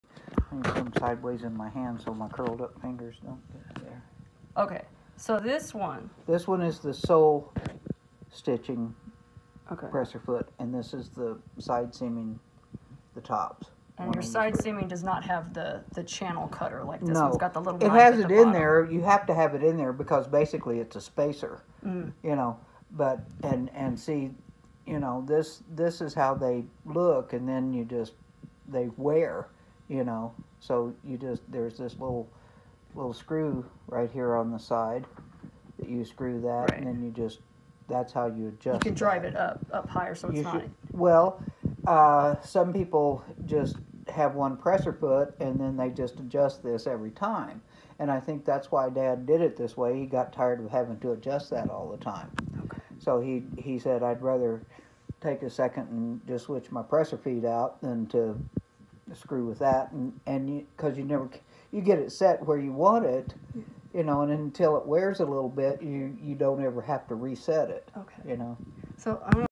Landis Curved Needle Stitcher Feet Bootmaking
With Deana McGuffin. Changing presser foot on Landis for side seaming cowboy boots vs sole stitching. I will continue to add references to help myself and anyone else interested in using these machines.